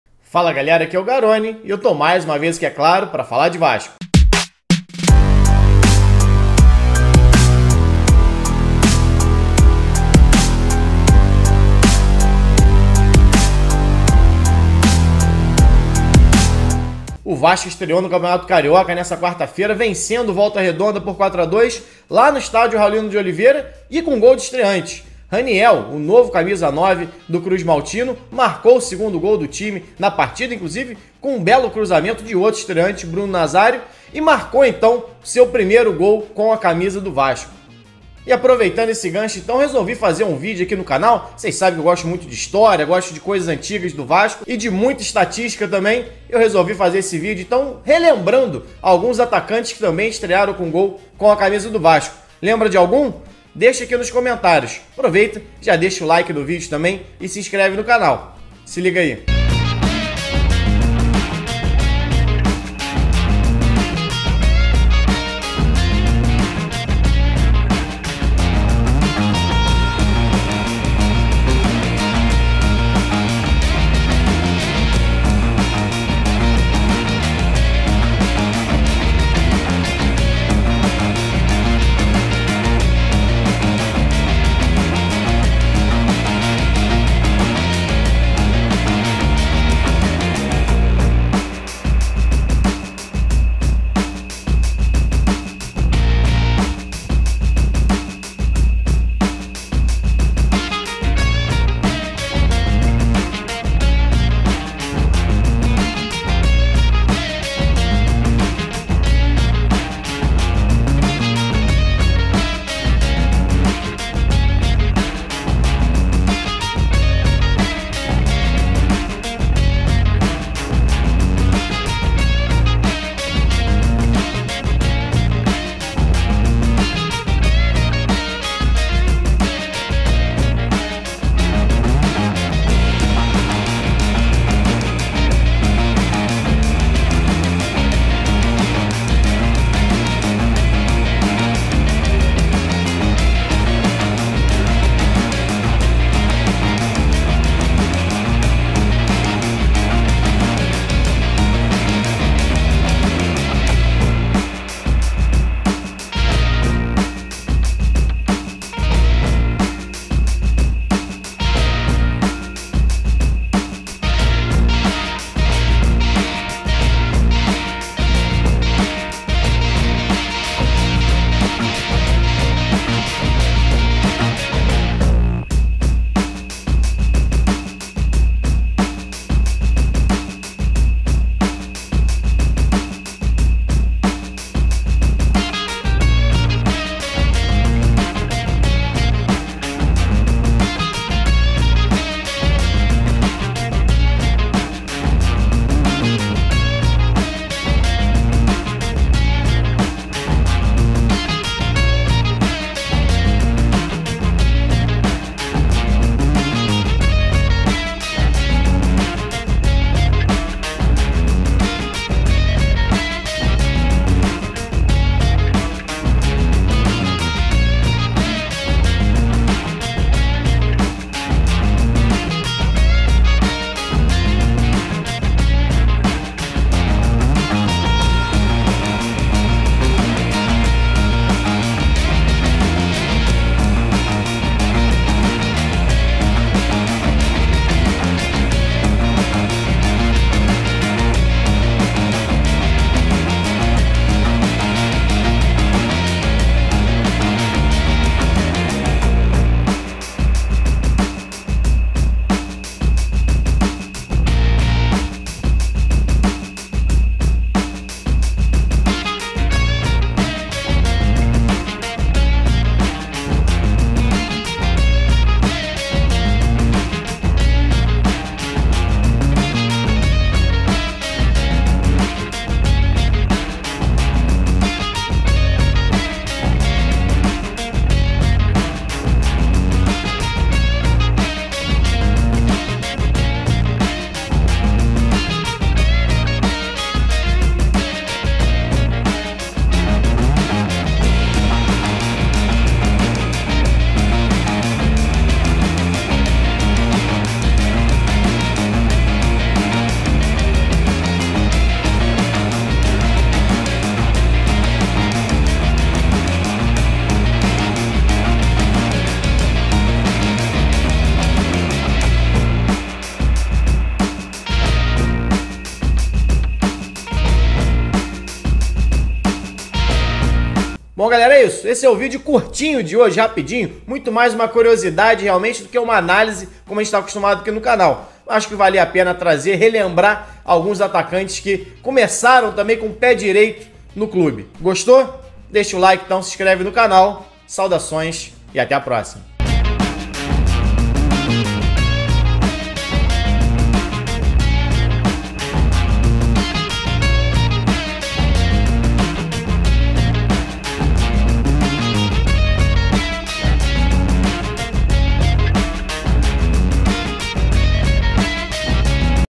RANIEL E A TRADIÇÃO DOS ARTILHEIROS DE ESTREIA NO VASCO
Raniel estreou com gol pelo Vasco. Atacante marcou o segundo na vitória por 4 a 2 sobre o Volta Redonda.
Relembre outros jogadores que estrearam com gol pelo Cruz-Maltino.